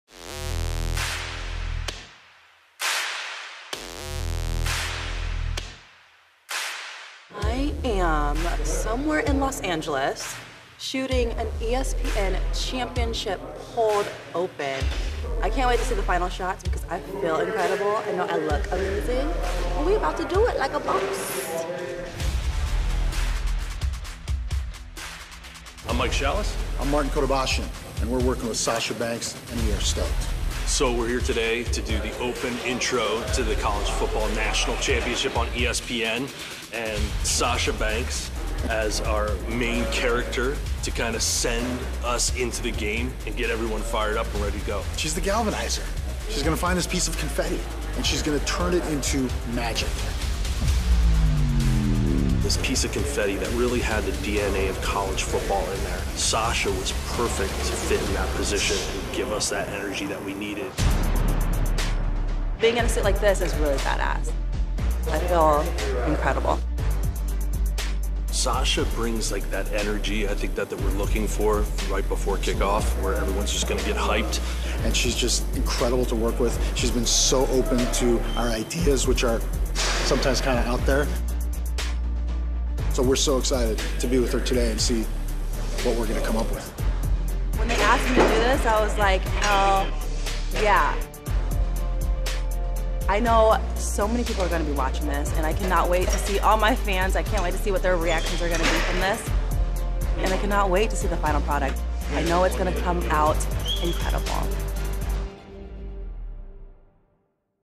Behind the scenes of Sasha Banks’ College Football National Championship intro on ESPN
Sasha Banks takes the WWE Universe behind the scenes of her special introduction for ESPN’s presentation of this year’s College Football National Championship game.